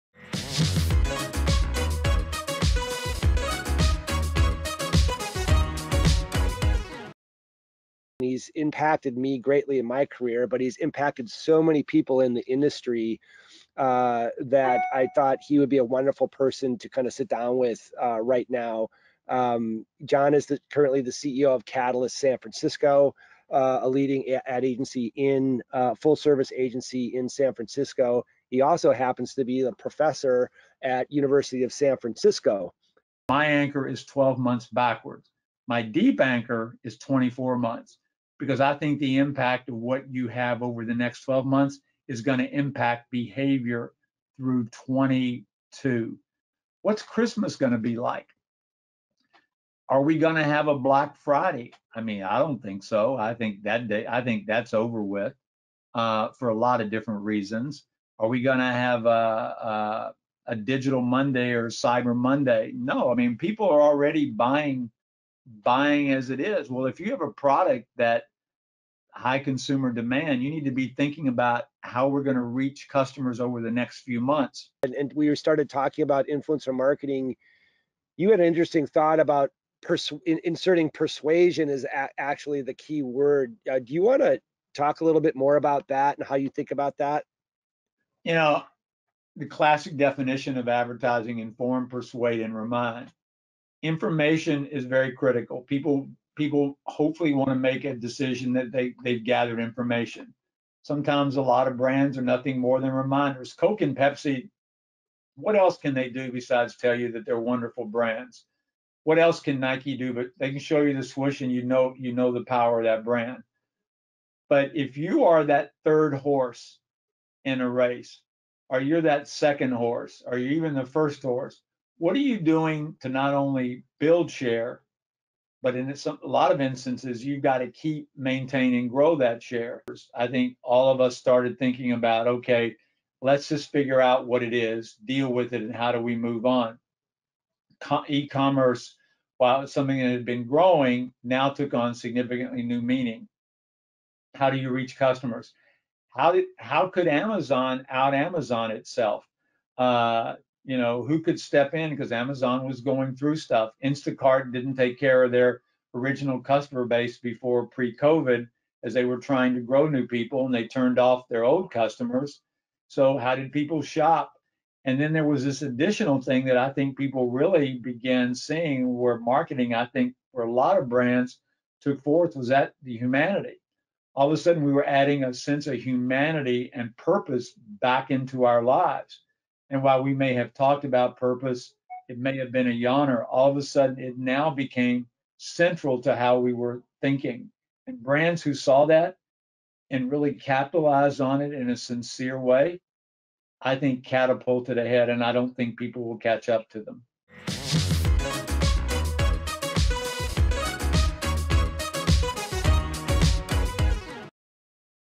Expert Series: Insights and Influence During a Time of Change
In Episode 4 of the Linqia Live Expert Series, we were joined by a special guest, John Durham, CEO of Catalyst SF and professor at USF. John had an immersive conversation with Linqia Chief Revenue Officer, Tim Richards, to discuss how brands should look forward and build a 90-day, 180-day and 12-month marketing plan.